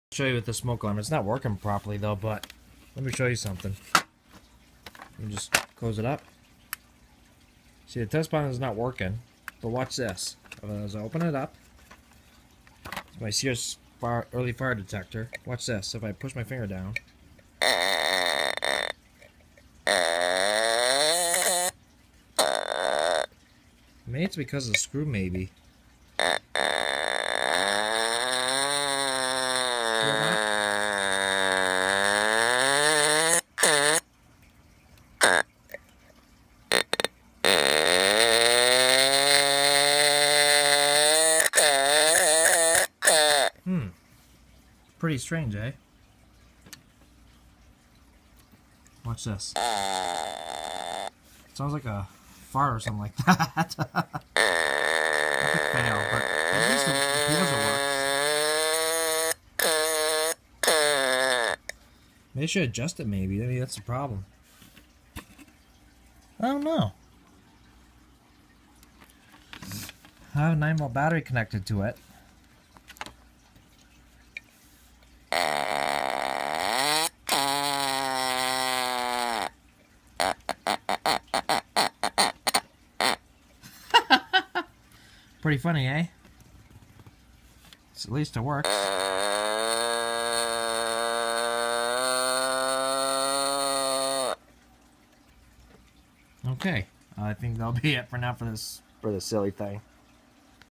EPIC FAIL! With my Sears "Early One" Fire Detector
I didn't realize that it needed a 9-Volt Alkaline battery... I did get one which you'll see in the next video.